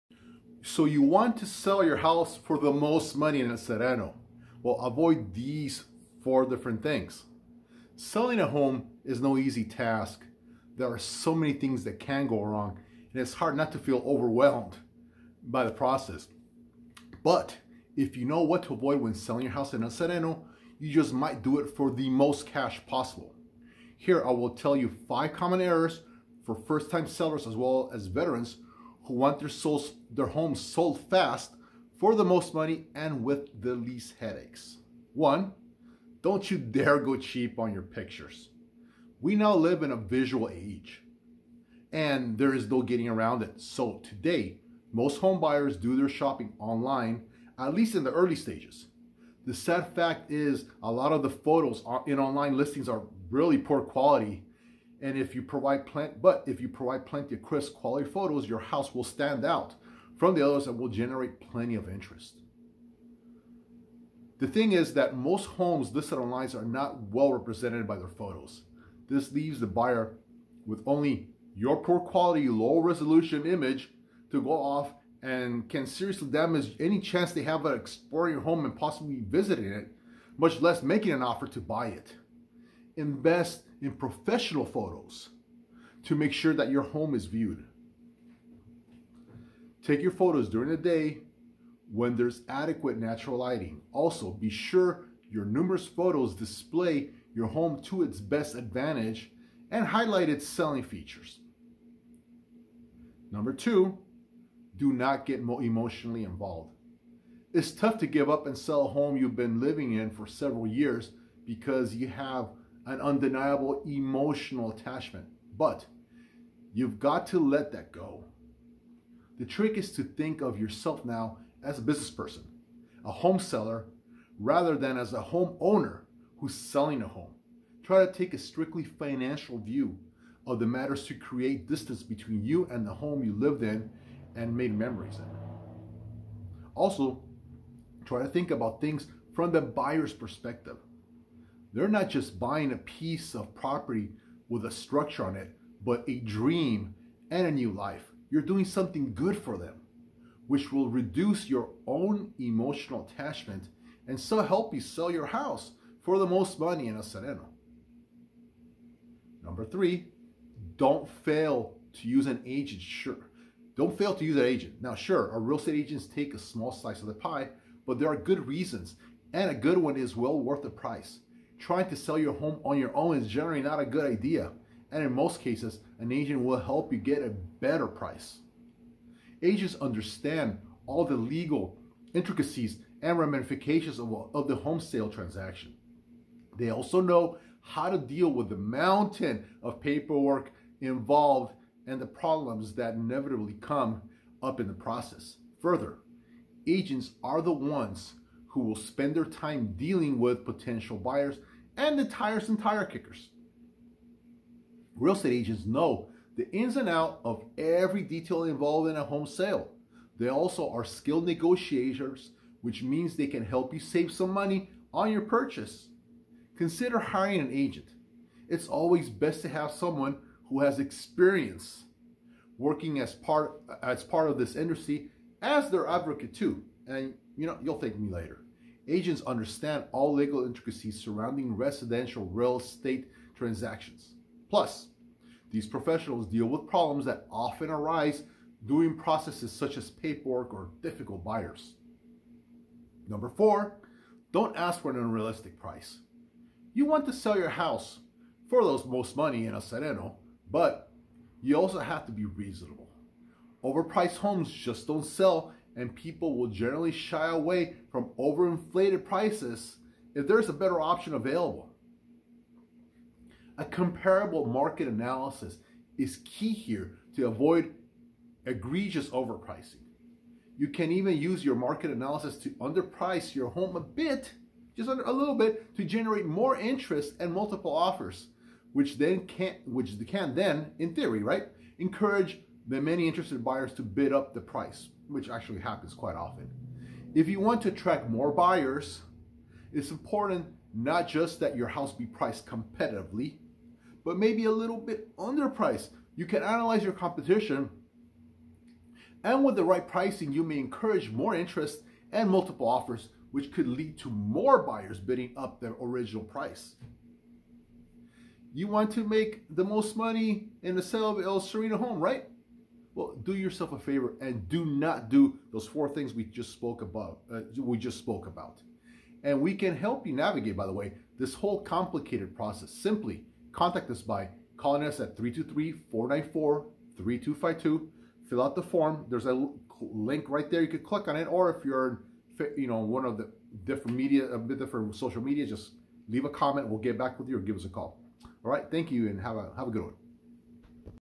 Want to Sell Your House for the Most Money in El Sereno 90032? Avoid These 4 Things
Selling a home is no easy task. There are so many things that can go wrong and it’s hard not to feel overwhelmed by the process.  But if you know what to avoid when selling your house in El Sereno you just might do it for the most cash possible! Here I will tell five common errors for first-time sellers as well as veterans who want their homes sold fast, for the most money and with the least headaches.